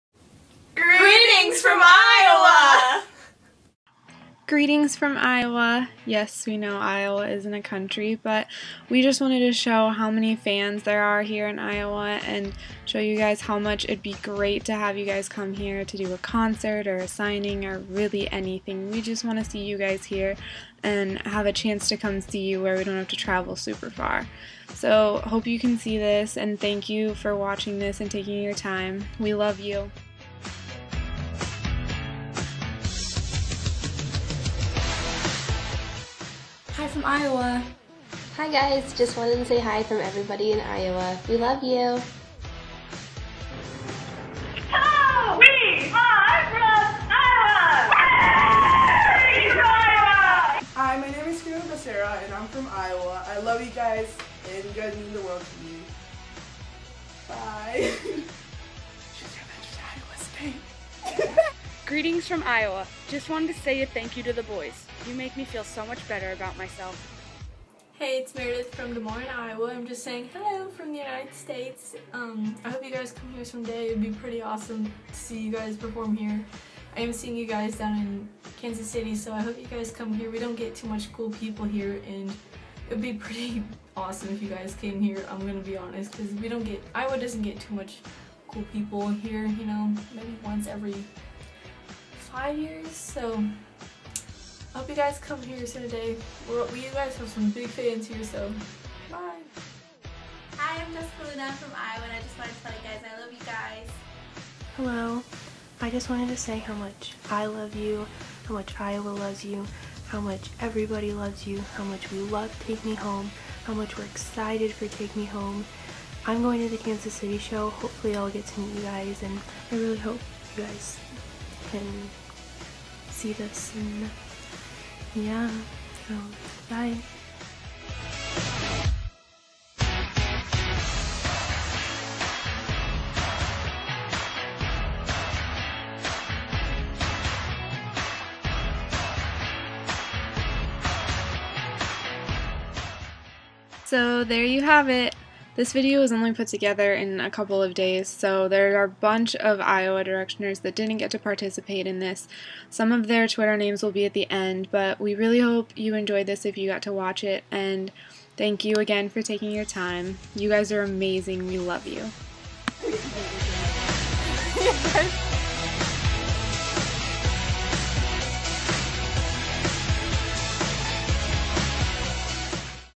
Greetings from America (Iowa): Bring 1D To Iowa
If you live in Iowa, you know it's not the most happening place. If you don't live in Iowa, you've probably flown over it. We're just a bunch of fans who want the chance to try and see/meet One Direction where we don't have to travel for hours and spend the money. This was made for the BringMeTo1D event at MSG in NY, but also for fun and to show the boys and other fans how much we love the boys and would love for them to stop by here!
P.S. They have 3 days between the Chicago & Minneapolis Take Me Home tour stops... =)

Put together by @LaineyC94
Follow @Official1DIA the Official Iowa Street Team on Twitter!! Those girls rock =)

Major shoutout and thanks to the Iowa girls who did/wanted to help with the video! Love you all! xx